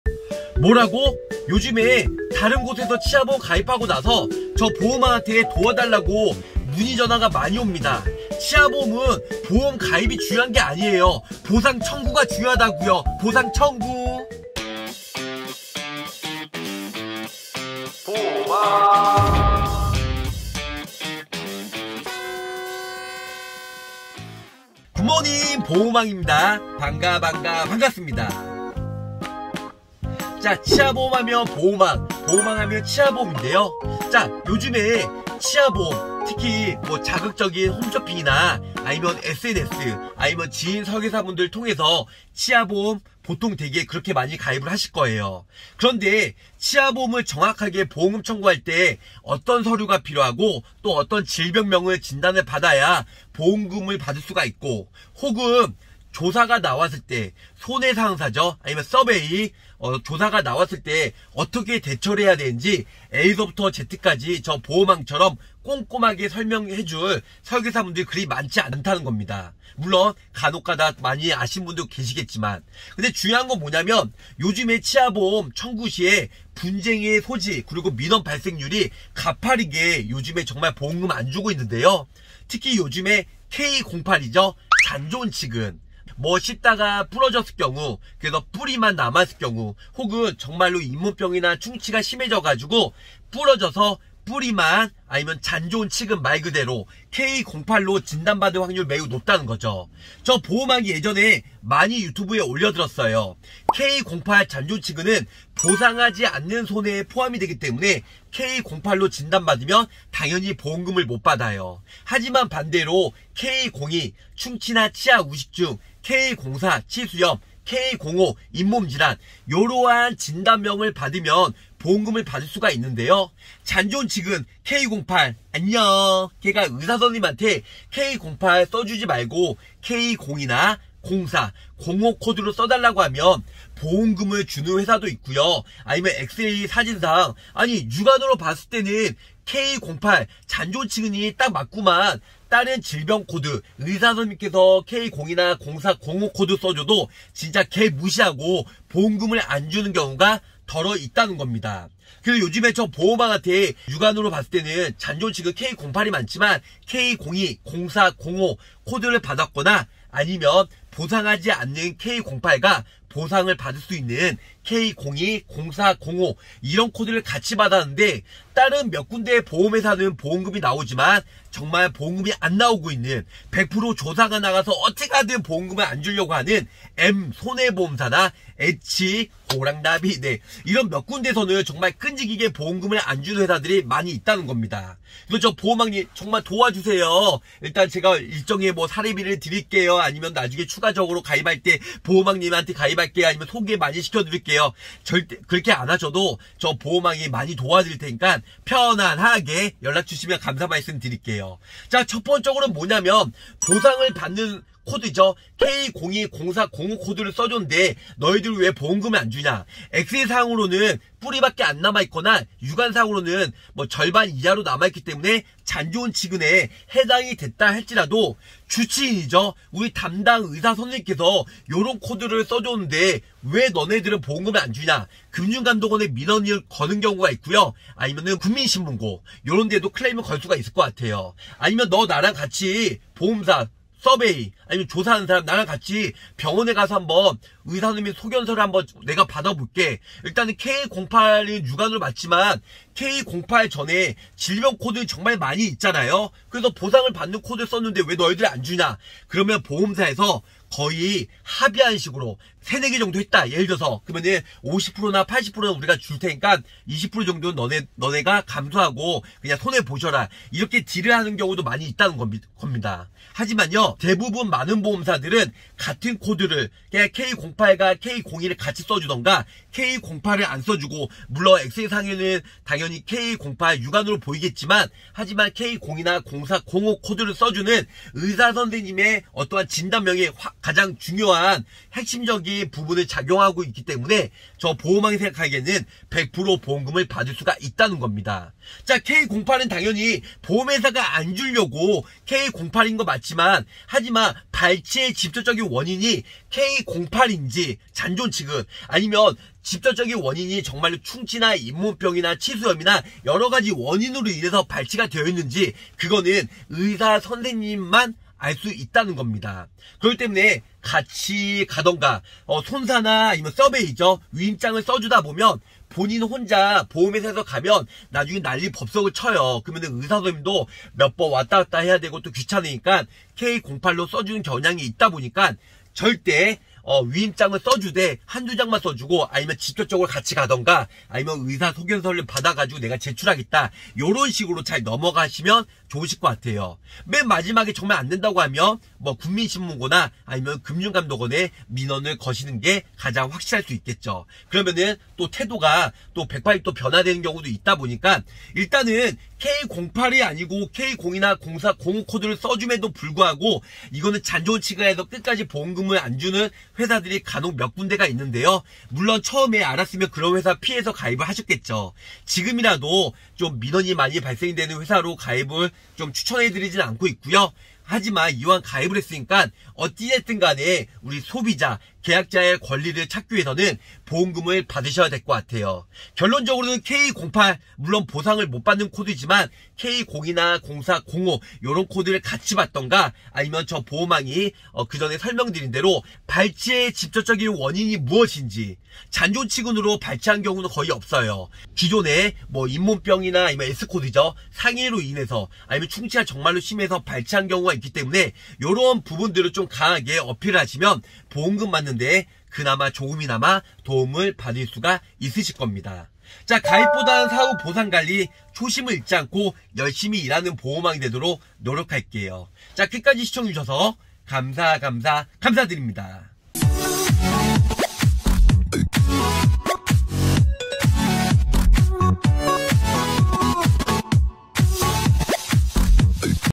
치아보험 분쟁많은 잔존치근(k08) 보상받는 꿀팁!! (툭하면 지급거절이야ㅠ)
분쟁많은 잔존치근(k08) 보상받는 꿀팁에 대해서

언제든지 문의주세요!!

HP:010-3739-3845
카톡친구추가:r2an
카톡플러스친구:보험왕